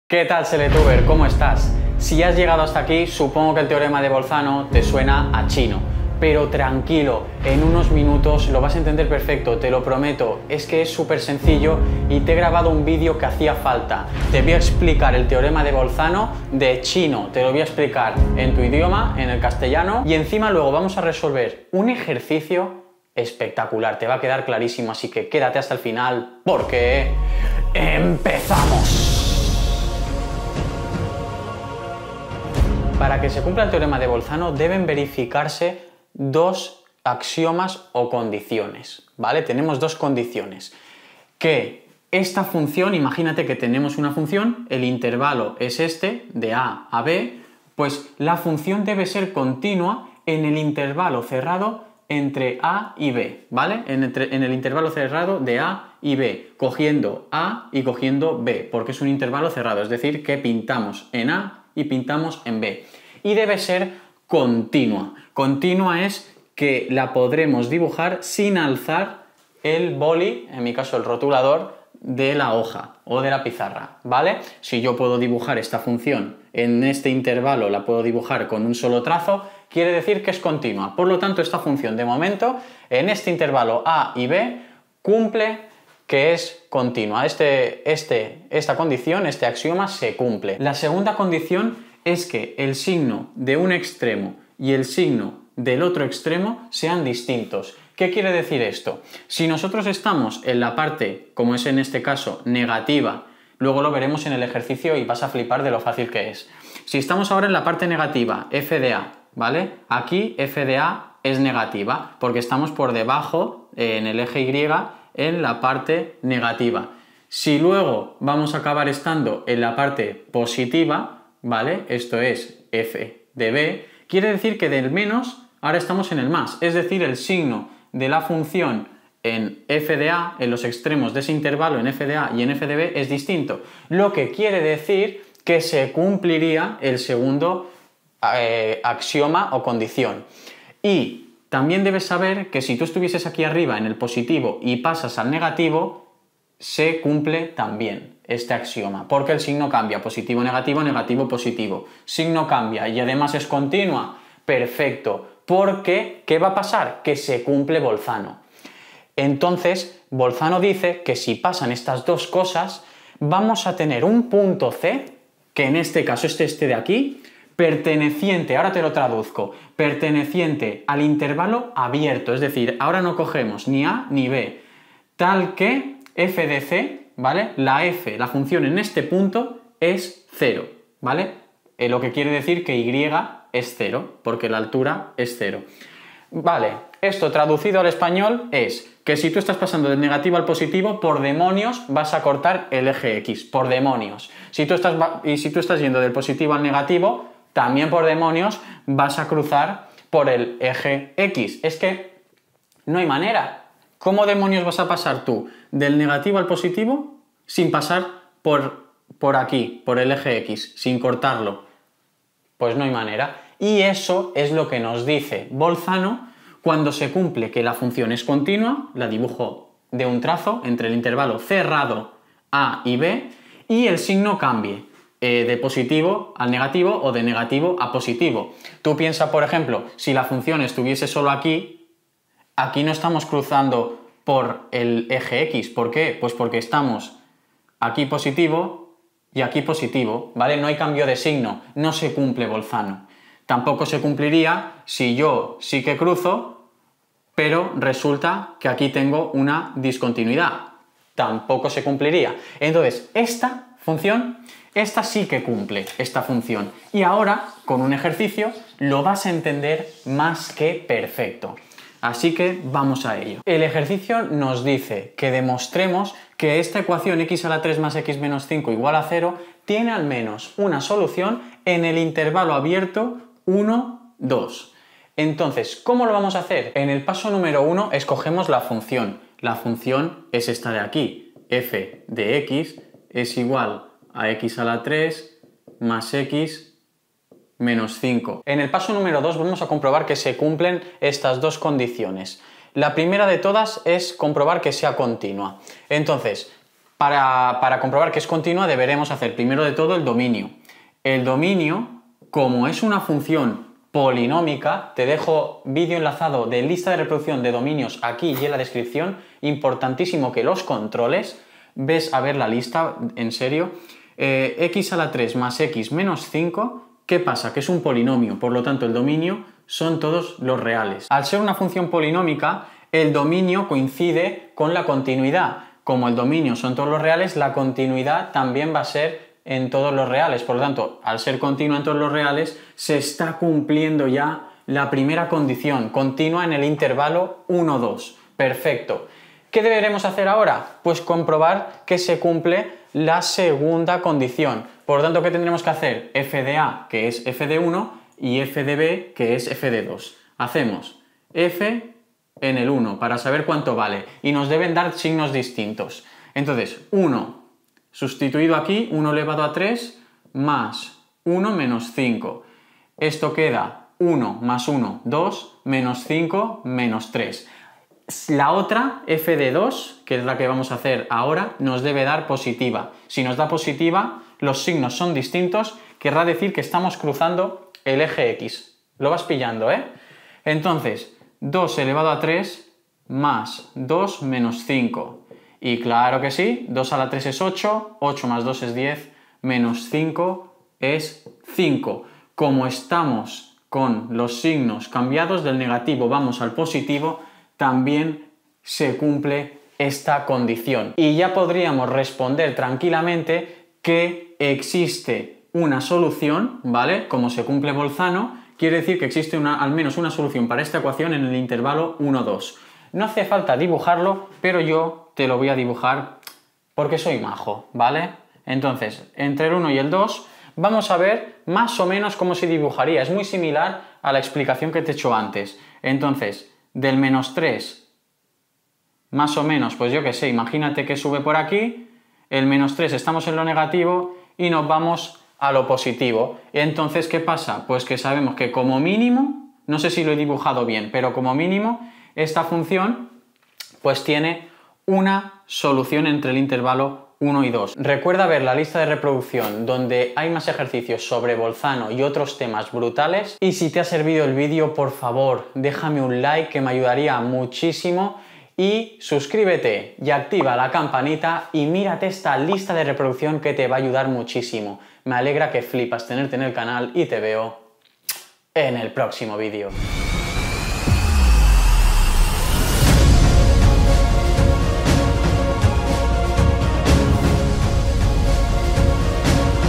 TEOREMA de BOLZANO [Desde CERO]
El teorema de Bolzano postula que si una función es continua en el intervalo cerrado [a,b] y además su signo cambia, es decir, el signo de f(a) es distinto al de f(b), existe al menos un punto c perteneciente al intervalo abierto (a,b) tal que f(c) = 0, o lo que es lo mismo, la función corta el eje x. Te voy a hacer la demostración junto con ejercicios resueltos.

// CONECTA CONMIGO //